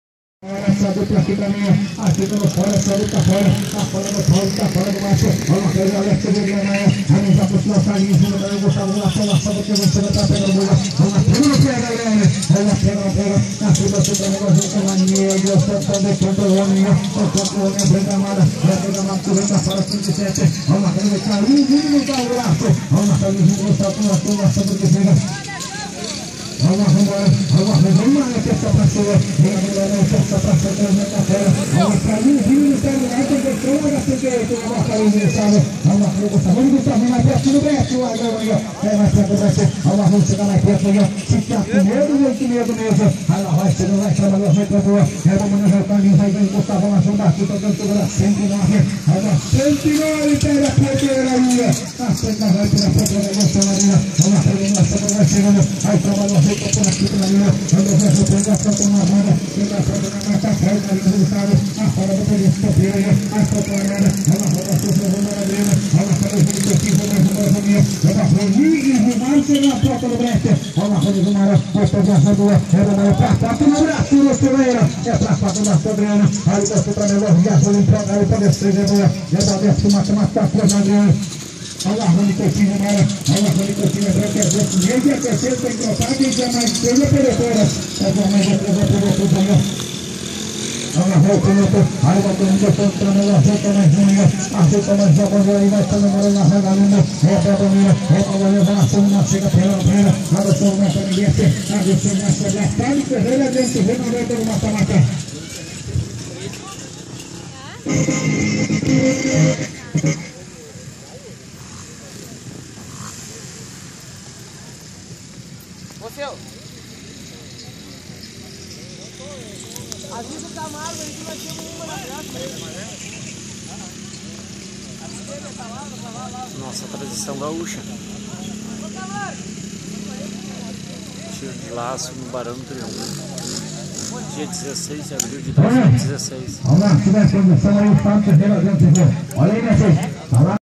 tiro de laco na serra do herval da 16-04-2016
estava passeando em parentes na regiao e fui ver no dia 16 de abril de 2016 a disputa pela taca do dia 16,04,16 na regiao do barao do triunfo proscimo onde eu estava passeando e fiz esse video de nossa tradicao gaucha no sul.